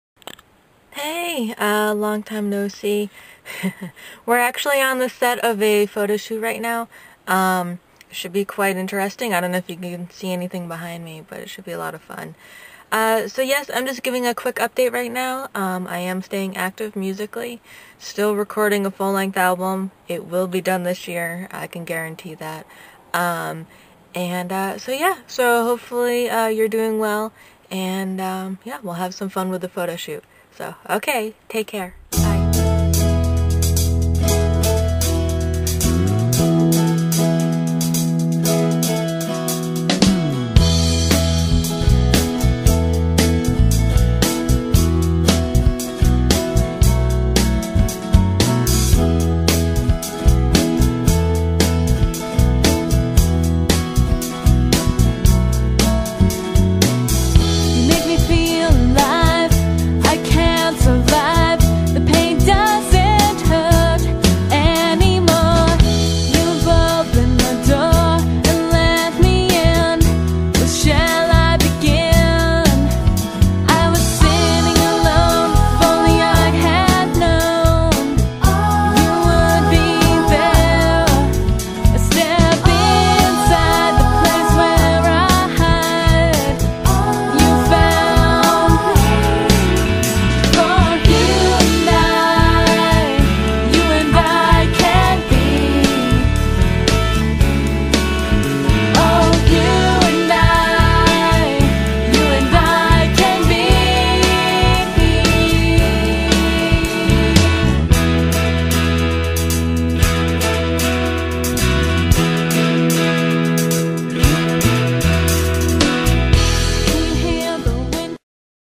Rachel Carson's Fall 2011 Photoshoot
Check out a quick update from Rachel, photos from her recent photoshoot, and hear a clip of a song from her debut album!

Photography by Rhonda Villanova.